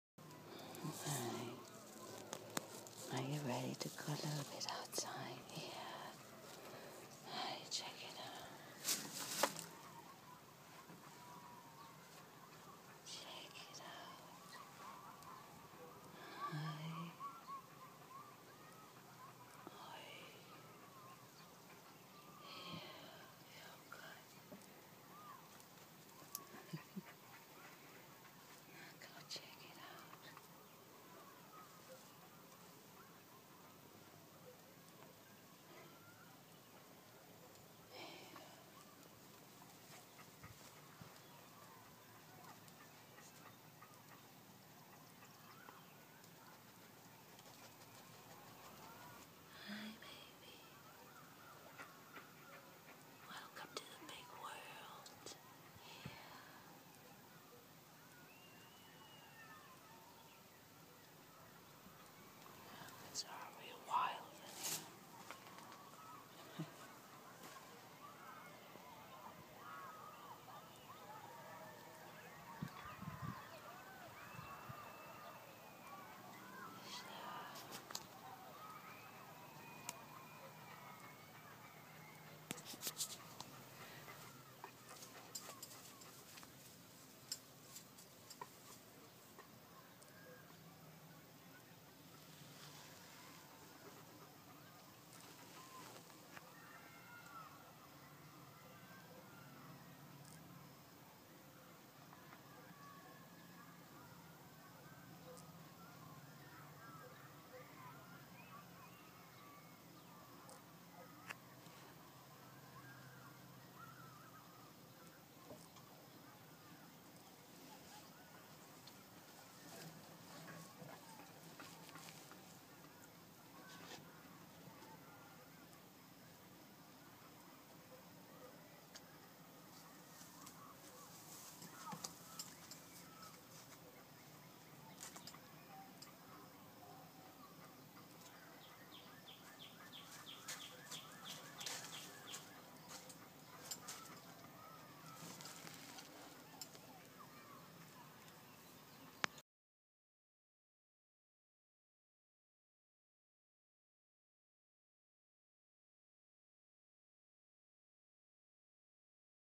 Orphaned Opossum is going outside for the first time
orphaned opossum raised at the Rainbow Wildlife Rescue